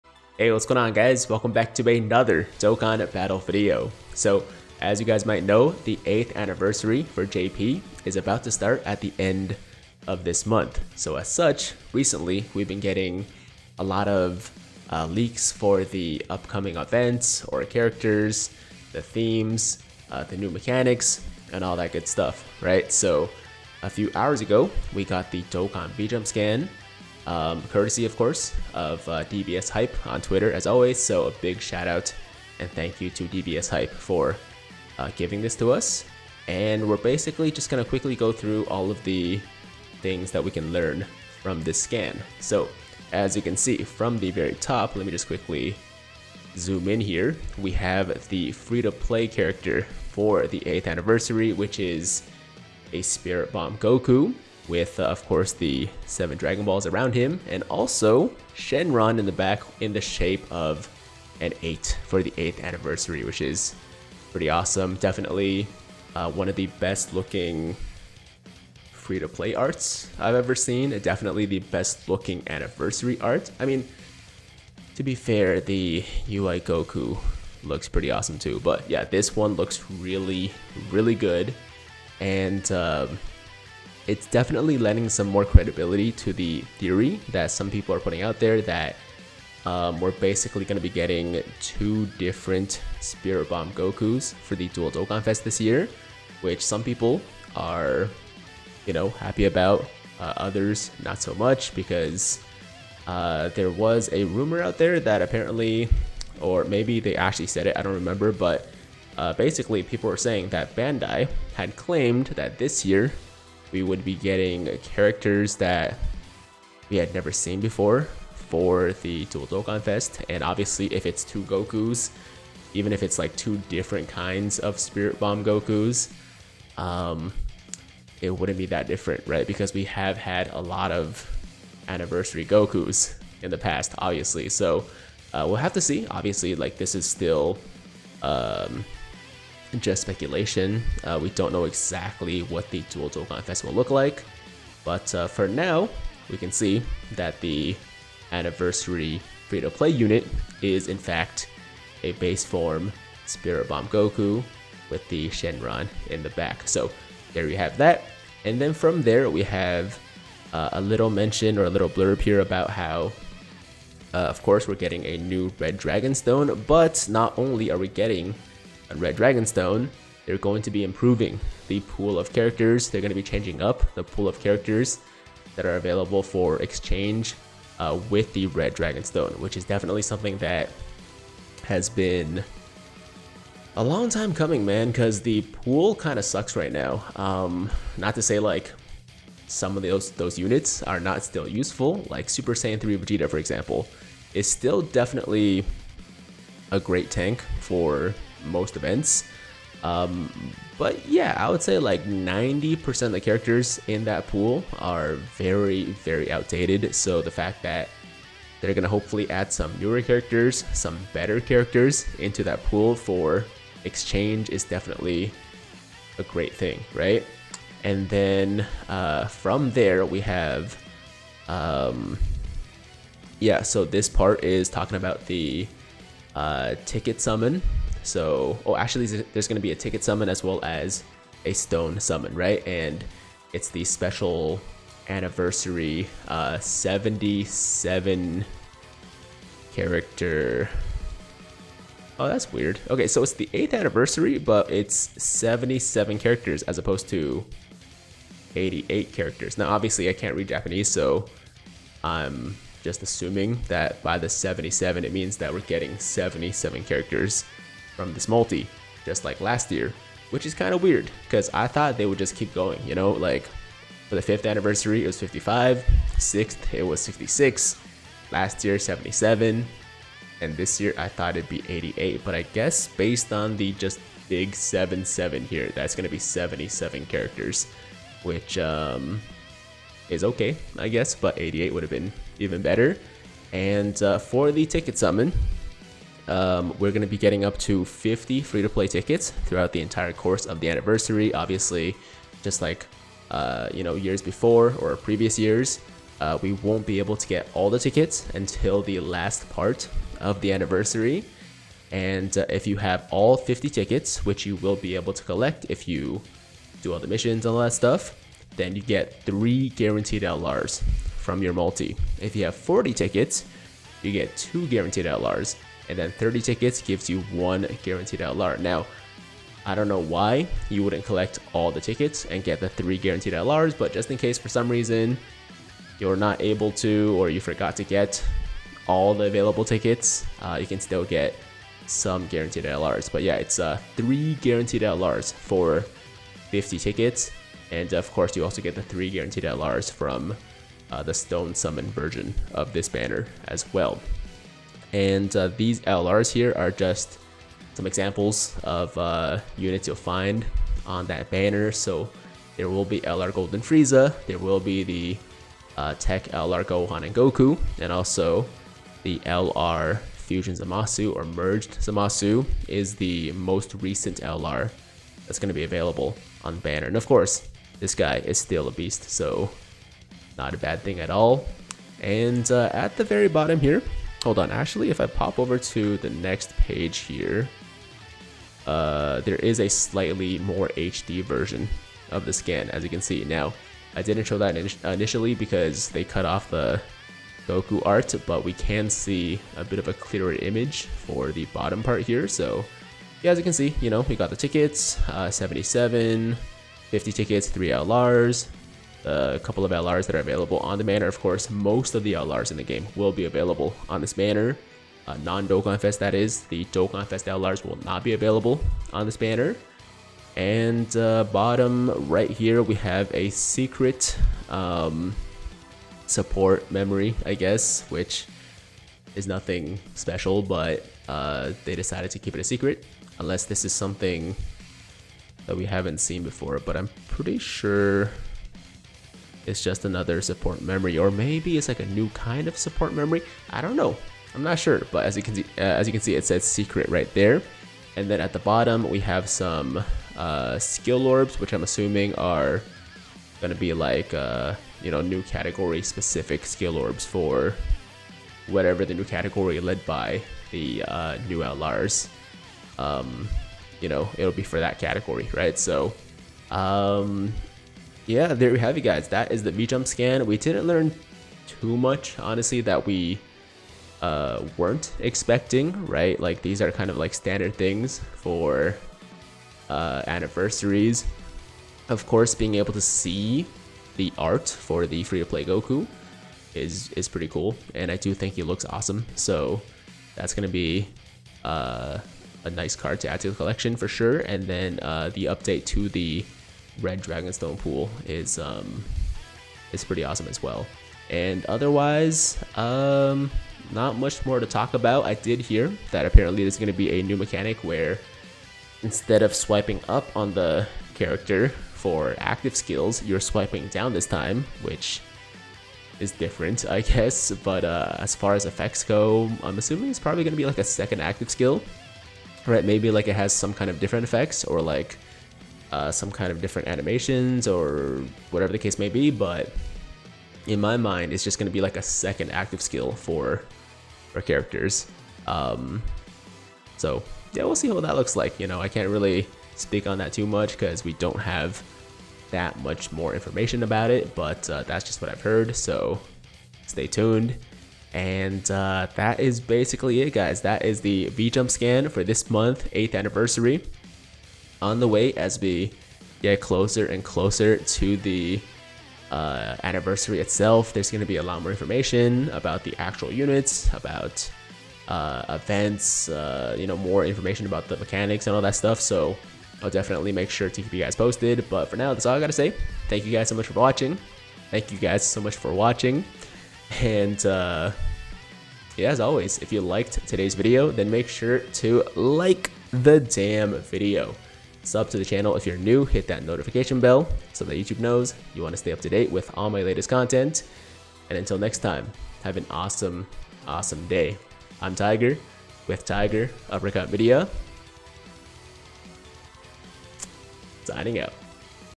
8TH ANNIVERSARY LEAKS ARE HERE! Dokkan V-Jump Scan Breakdown (January 2023)
Here's EVERYTHING we learned from the latest Dokkan V-Jump scan!